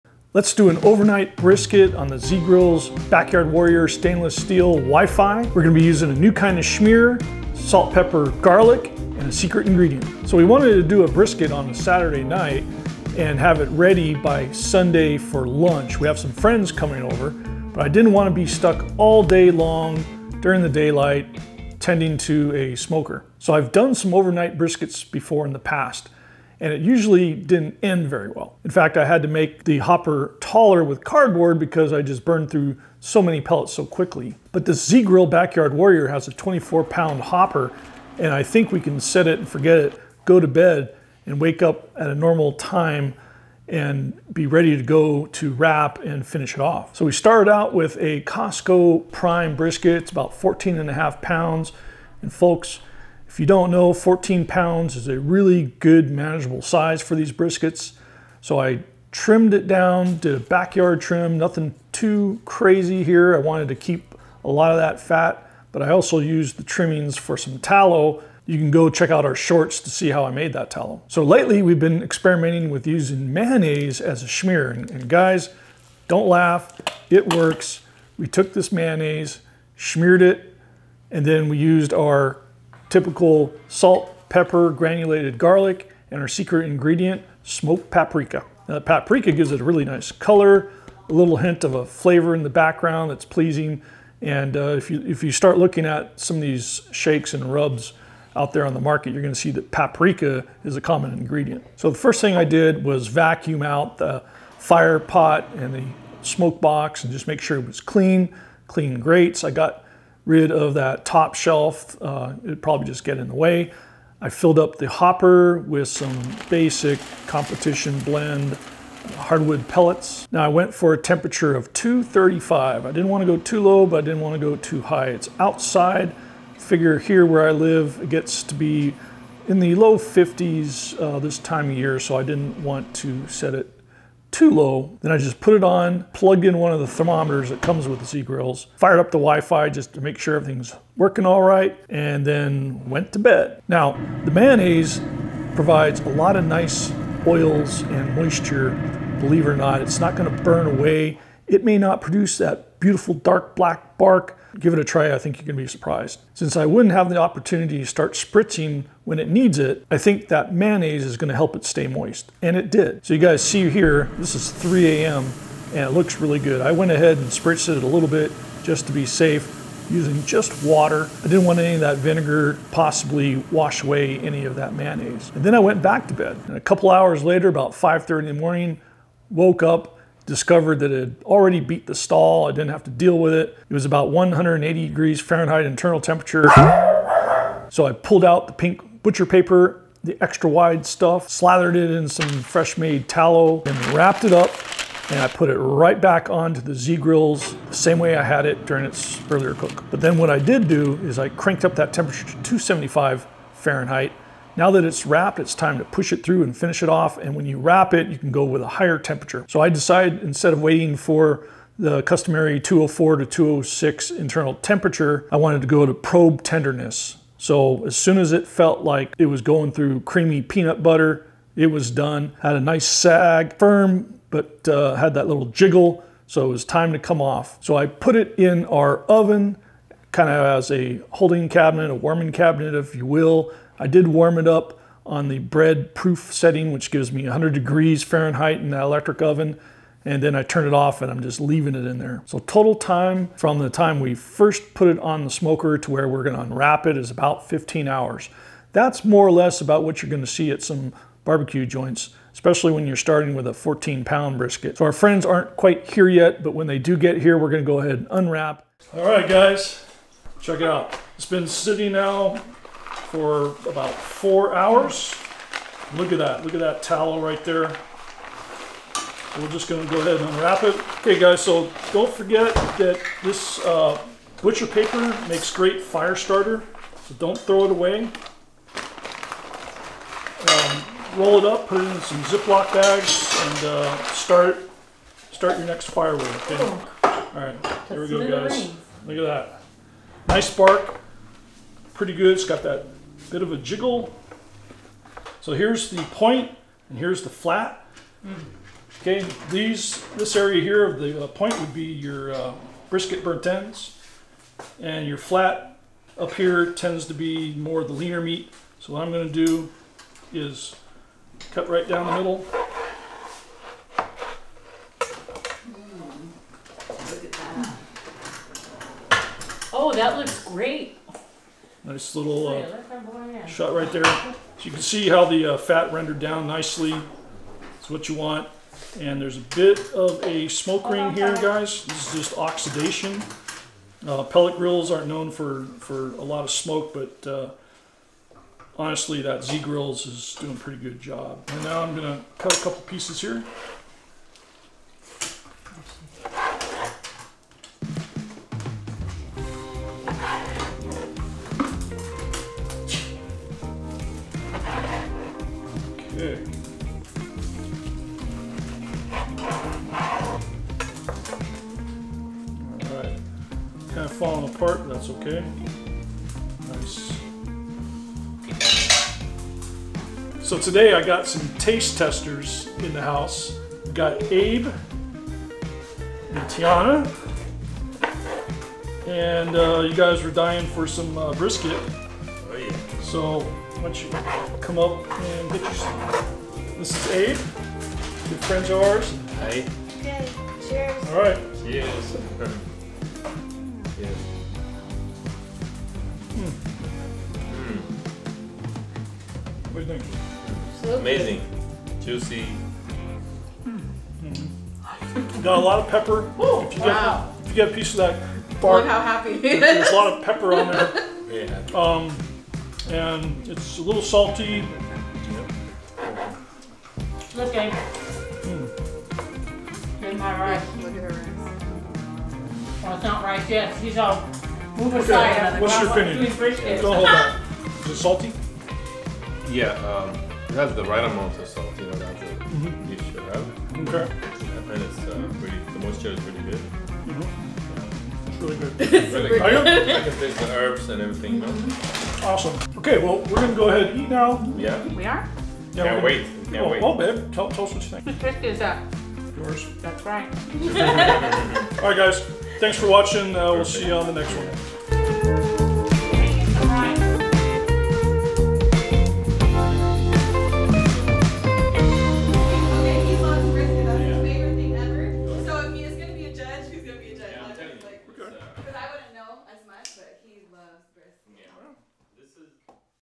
Overnight ZGrills pellet smoker brisket | Mayonnaise binder experiment
We love our ZGrills 700 series Backyard Warrior Stainless Steel, and we also love brisket. But hate having to set around all day cooking it. SO the next best thing is to "set it and forget it" at night so we can wake up to this tasty treat. Lets show you how we did!

Our GreenhornBBQ Amazon Store:

Special Deals direct from Z Grills!
Featured is the Z Grills 700 series Backyard Warrior WiFi + Stainless Steel pellet grill review.